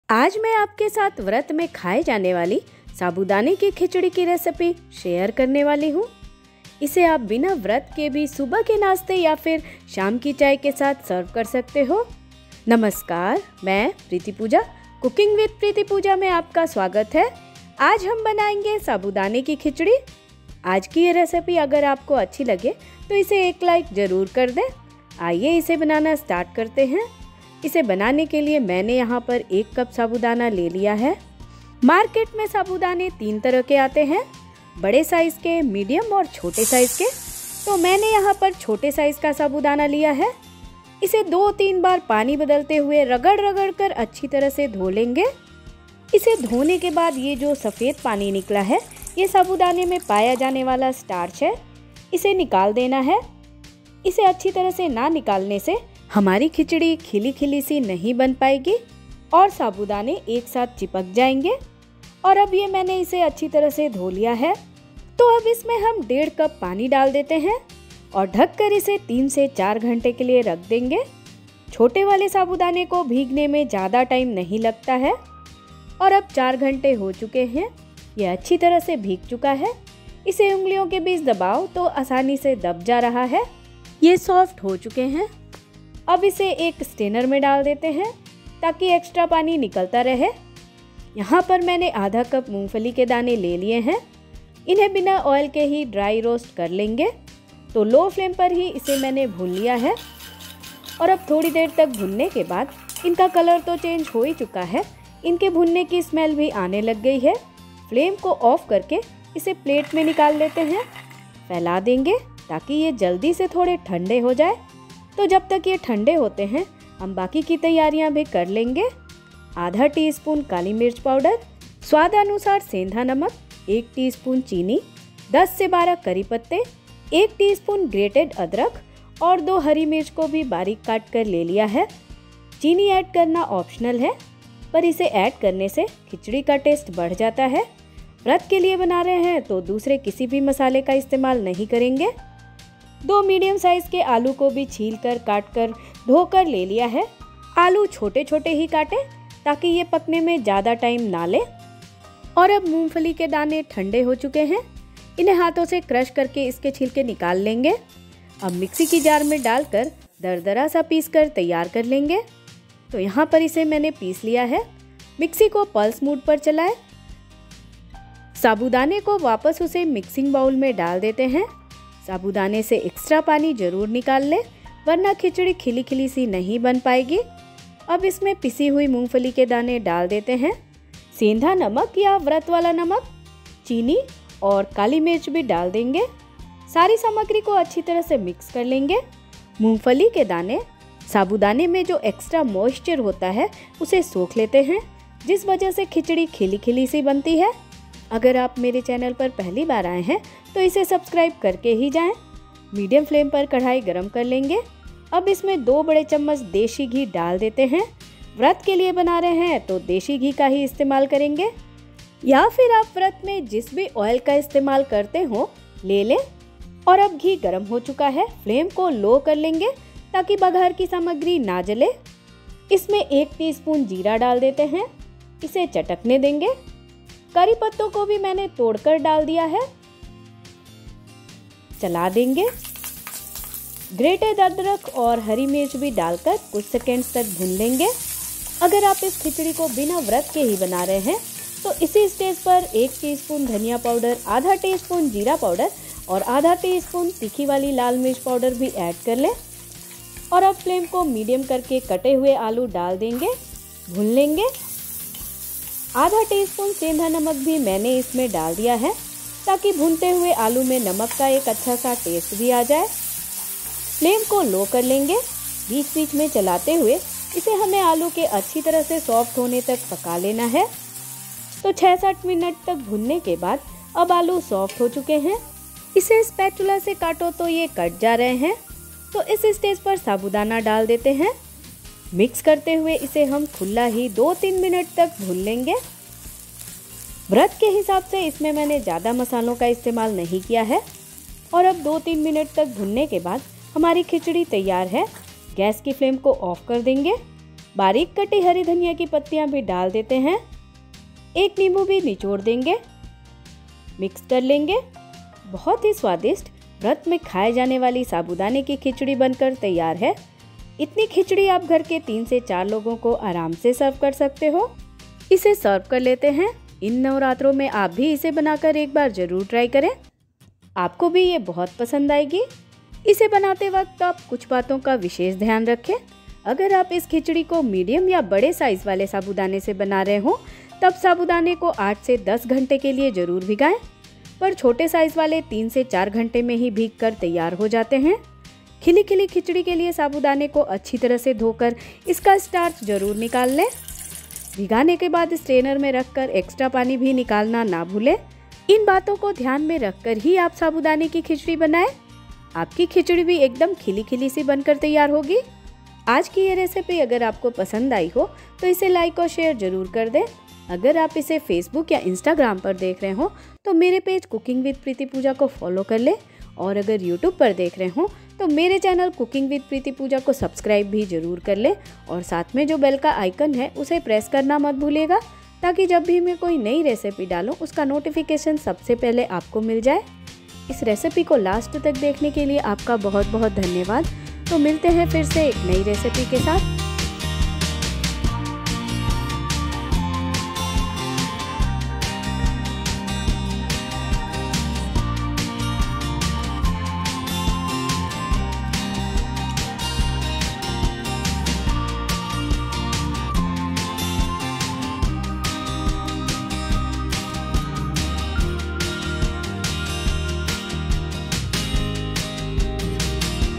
साबूदाना खिचड़ी बनाने की विधि | How to Make Sabudana Khichdi | Vrat Recipe
Sabudana Khichdi is a popular Indian breakfast and snack recipe that is made using tapioca pearls, peanuts, potatoes, and a few spices. In this video, we will show you step-by-step how to make this delicious and nutritious dish at home.

We will start by soaking the sabudana pearls in water and then draining them after a few hours. Then, we will roast some peanuts and chop them into small pieces. Next, we will peel and dice some potatoes and fry them until they are golden brown.

Once these ingredients are ready, we will mix them together with some green chillies, cumin seeds, curry leaves, and salt. We will then cook the mixture on a low flame for a few minutes, stirring occasionally, until the sabudana pearls become transparent.

Finally, we will garnish the sabudana khichdi with some freshly chopped coriander leaves and serve it hot with some curd or chutney. Whether you are looking for a quick and easy breakfast or a healthy snack, this sabudana khichdi recipe is sure to delight your taste buds!

साबूदाना खिचड़ी -
साबूदाना - 1 कप
मूंगफली के दाने - ½  कप
आलू - 2 मीडियम साइज़
हरी मिर्च - 2
ग्रेटेड अदरख - 1 Tsp
काली मिर्च पाउडर - ½ Tsp
सेंधा नमक - स्वादानुसार
चीनी - 1 Tsp
देशी घी - 2 Tbsp
जीरा - 1 Tsp
करी पत्ता - 10-12
बारीक कटी हरी धनिया की पत्तियाँ - 3-4 Tsp
निम्बू - 1

Sago pearls - 1 Cup
Peanut - ½ Cup
Potato - 2  Medium sized
Rock Salt - According to taste
Sugar - 1 Tsp
Deshi ghee - 2 Tbsp
Finely chopped green coriander leaves - 3-4 Tsp
Lemon - 1